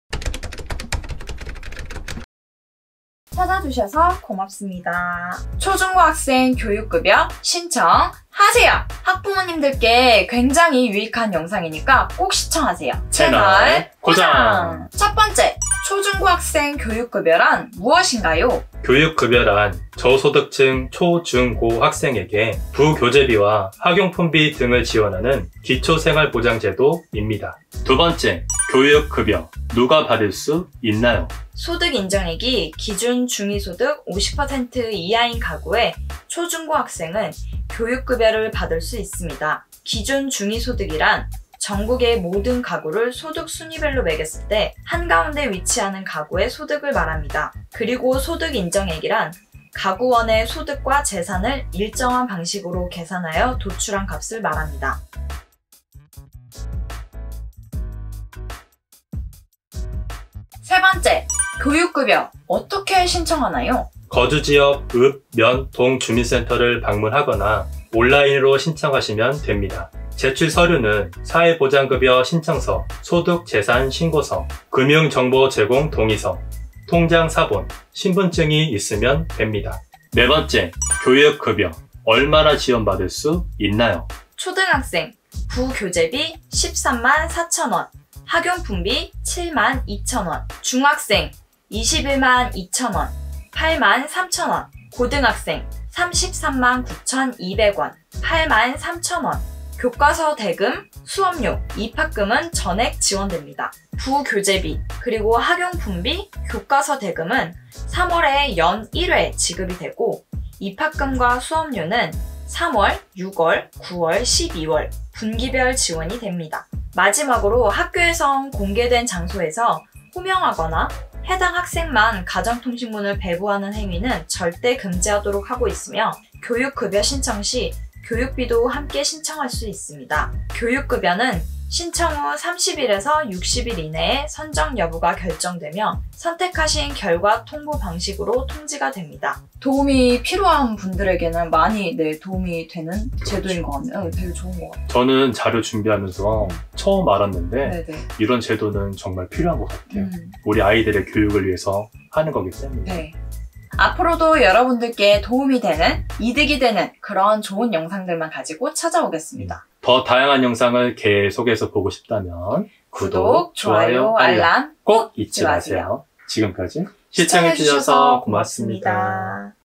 교육급여에 모든 것 ! 초중고 학생 '교육급여' 신청하세요! 많은 혜택이 들어있는 시간! 학부모님들 필수영상!
교육급여에 모든 것 ! 초중고 학생 '교육급여' 신청하세요!
많은 혜택이 들어있는 시간! 학부모님들 필수영상!